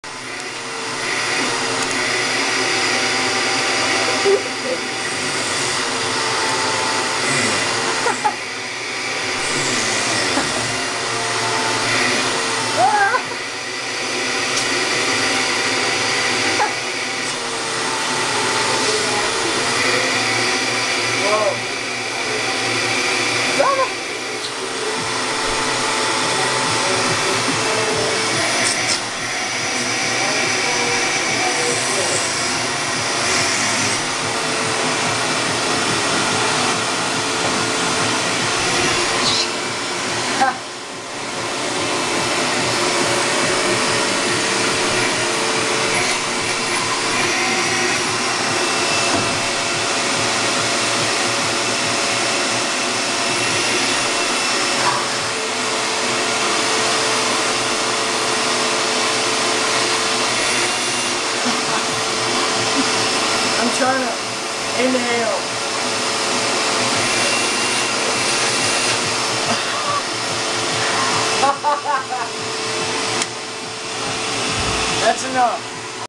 vaccum sucking your face
This was in the 8th grade and Wesley vaccum sucked his face haha